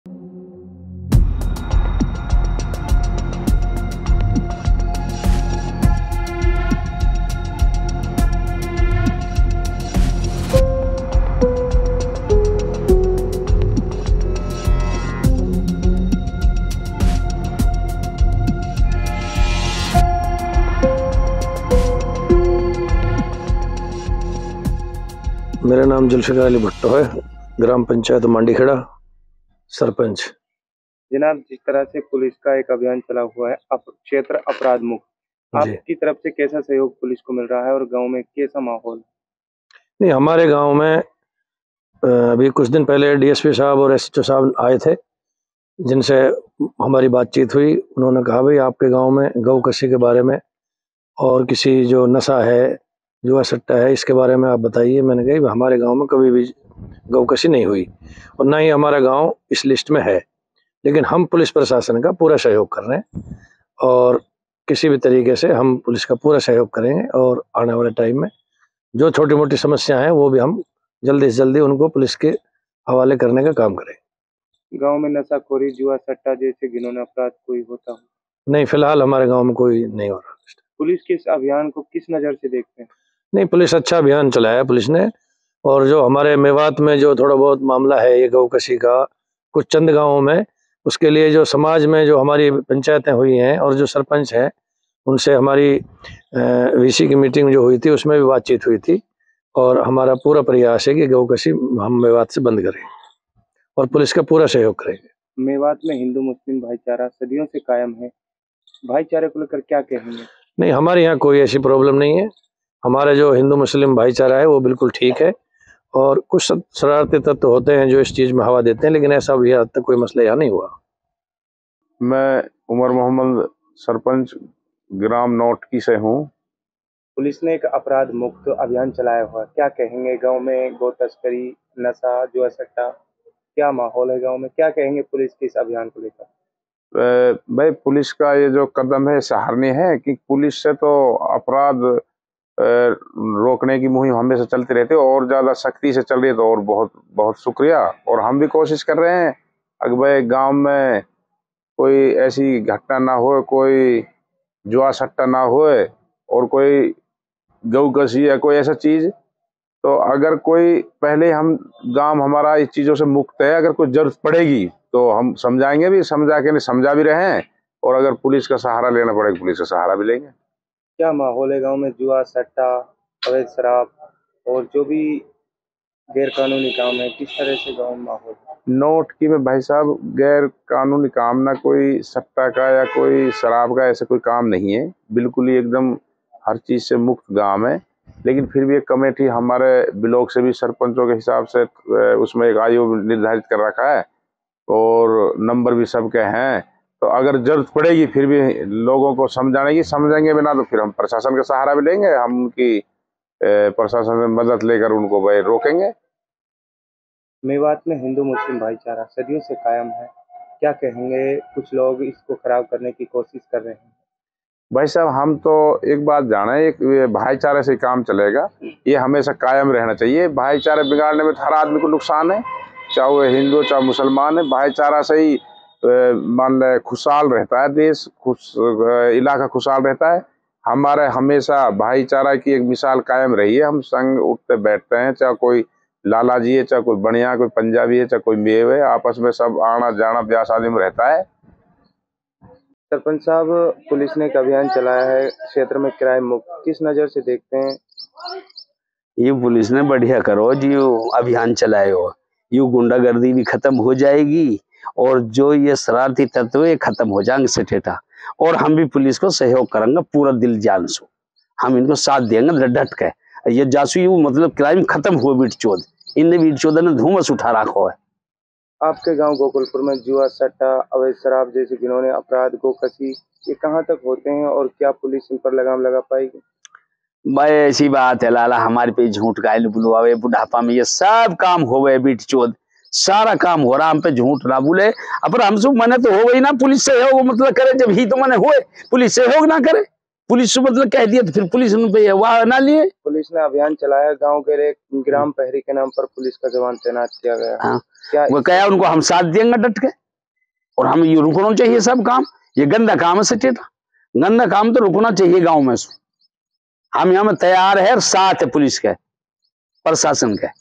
Nuh News | अब सरपंच भी रखेंगे गौ तस्करों पर पैनी नज़र | News18 Haryana
Nuh News | पुलिस का अपराध मुक्त अभियान का सहयोग करने के लिए मेवात जिले के सरपंचों ने कमर कस ली है । मेवात जिले के सरपंचों का कहना है कि अब हर गांव से सरपंच पुलिस का सहयोग करेंगे और गांव को अपराध मुक्त बनाएंगे गांव में गोकशी अवैध शराब जुआ सट्टा जैसे घिनौने अपराधों को नहीं होने दिया जाएगा ।